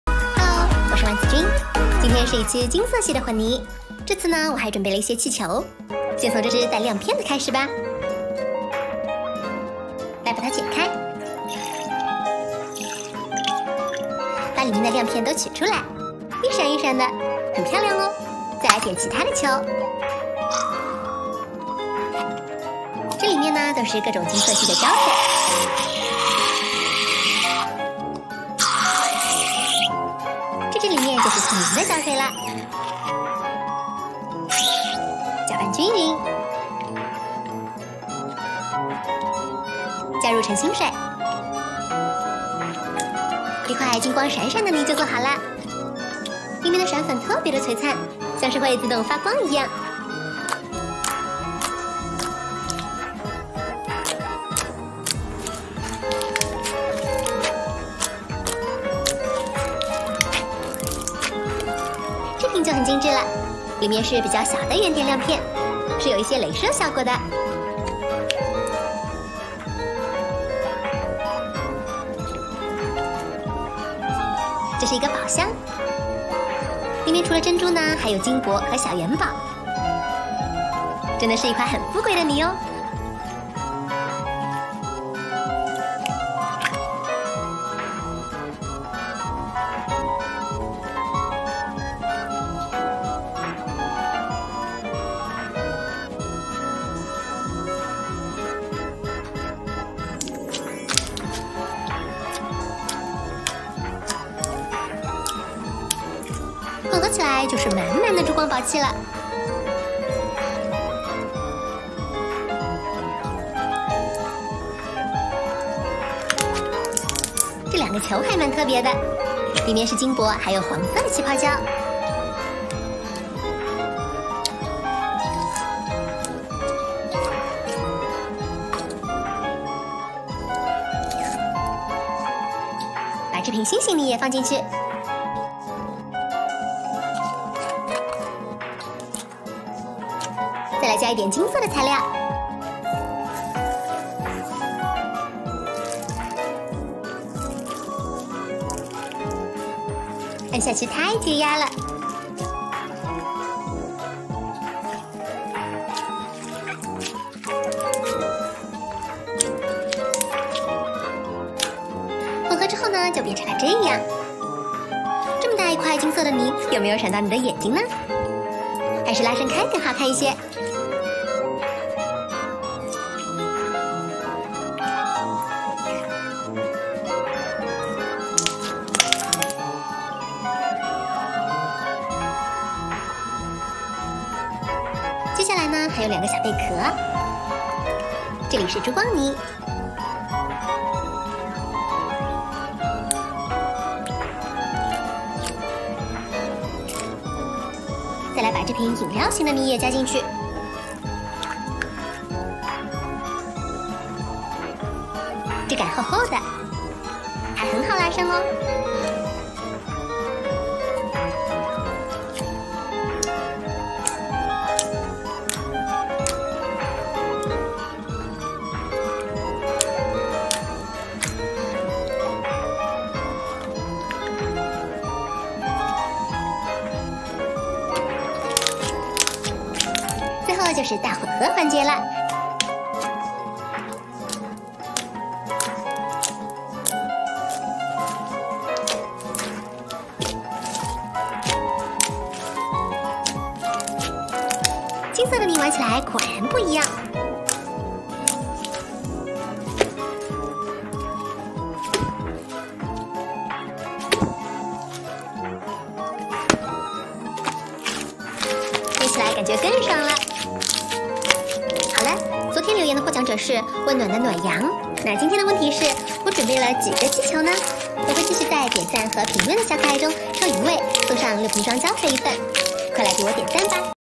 【玩子君手作】 金色史莱姆大集合！金色气球加上金色的无硼砂泥，也太土豪了！
手工教程，创意手工，亲子手工
►金色史莱姆大集合！金色气球加上金色的无硼砂泥，也太土豪了！
超梦幻的无硼砂双拼混泥，阳光彩虹玩出新花样
💗大家好，请支持玩子君手作, 帮助我获得1.000位关注者, 谢谢大家🥰🥰🥰